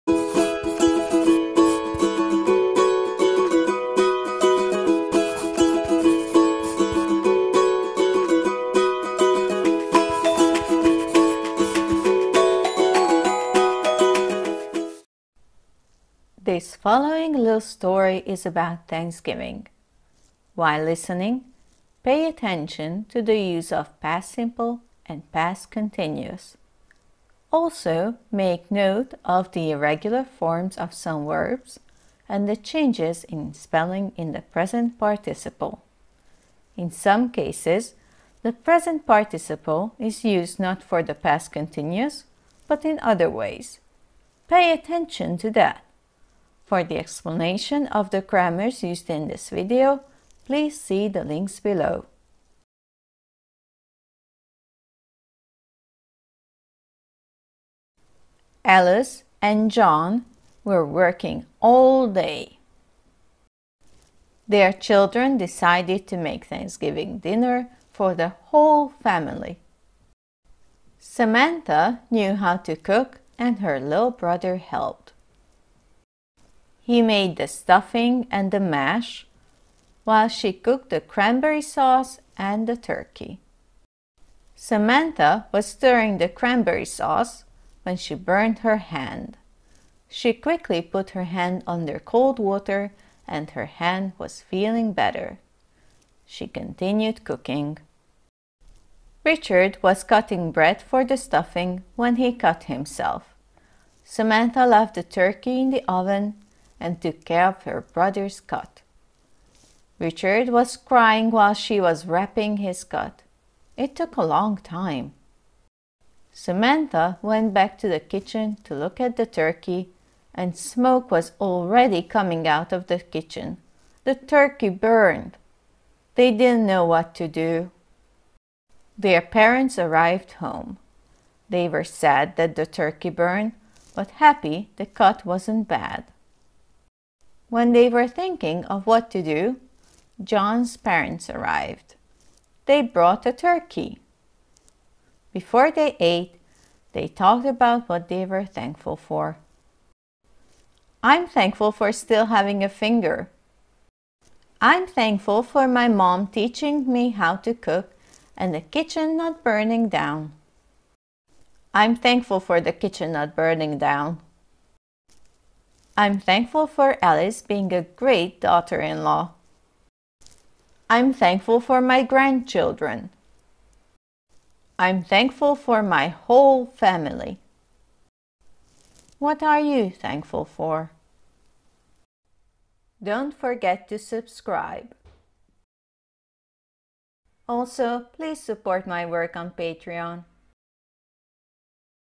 A Thanksgiving Story | English for Adults
The following little story is about Thanksgiving. While listening, pay attention to the use of Past Simple and Past Continuous. Also, make note of the irregular forms of some verbs and the changes in spelling in the Present Participle. In some cases the Present Participle is used not for the Past Continuous, but in other ways. Pay attention to that!

If you become my Patron, you can download this as a PDF!